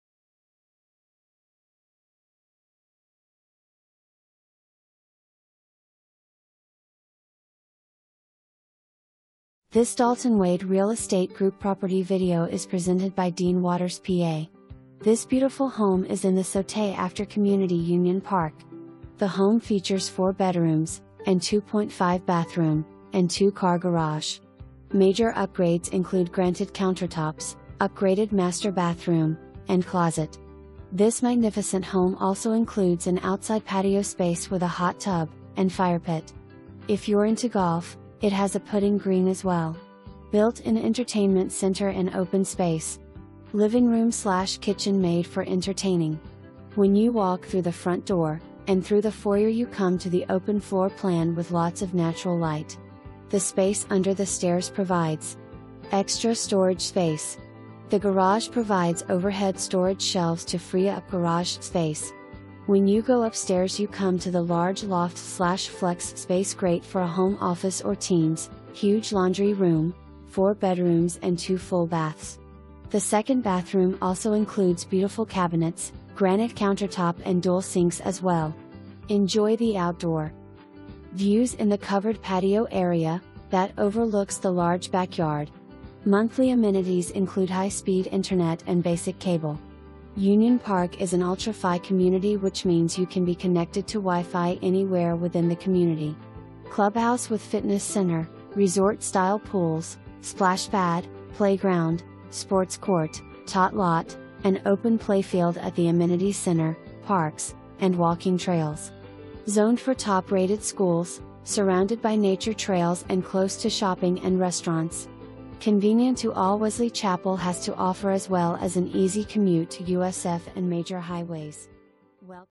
Dalton Wade Real Estate Group - 1652 LUDINGTON AVENUE
This beautiful home is in the sauté after community Union Park. The Home features 4 bedrooms, and 2.5 bathroom, and 2 car garage. Major upgrades include granted countertops, upgraded master bathroom, and closet. This magnificent home also includes an outside patio space with a hot tub, and firepit. If you’re into golf, it has a putting green as well. Built in entertainment center in open space. Livingroom/kitchen made for entertaining. When you walk through the front door, and through the foyer you come to the open floor plan with lots of natural light. The space under the stairs provides. extra storage space. The garage provides overhead storage shelves to free up garage space. When you go upstairs you come to the large loft/flex space great for a home office or teens, huge laundry room, 4 bedrooms and 2 full baths. The second bathroom also includes beautiful cabinets, granite countertop and dual sinks as well. Enjoy the outdoor
views in the covered patio area, that overlooks the large backyard. Monthly amenities include High Speed Internet and basic cable. Union Park is an Ultra-Fi community which means you can be connected to WIFI anywhere within the community. Clubhouse with fitness center, resort style pools, splashpad, playground, sports court, tot lot, an open playfield at the amenity center, parks, and walking trails. Zoned for Top rated schools, surrounded by nature trails and close to shopping and restaurants. Convenient to all Wesley Chapel has to offer as well as an easy commute to USF and major highways. Welcome home! Schedule your showing today.

2 full bath, 1 half bath

Dean Waters PA
Dalton Wade Real Estate Group